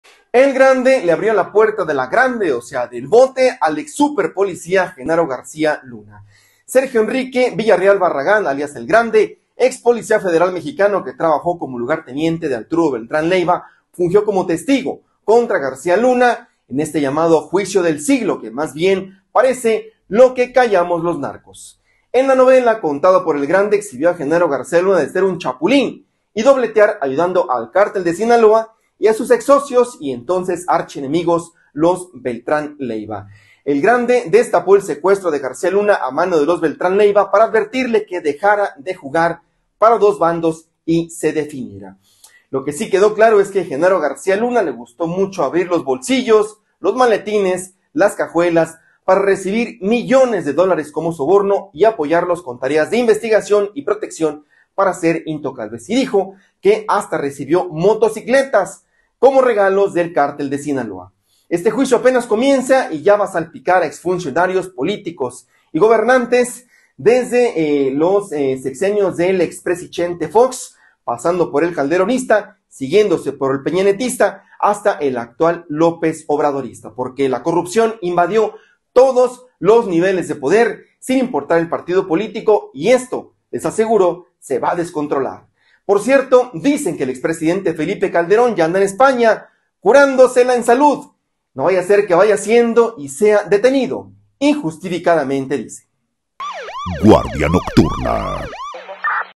Noti Reel
Destapan el secuestro de García Luna a manos de los Beltrán Leyva, habría sido una advertencia para que dejara de “dobletear”, ayudando al Cártel de Sinaloa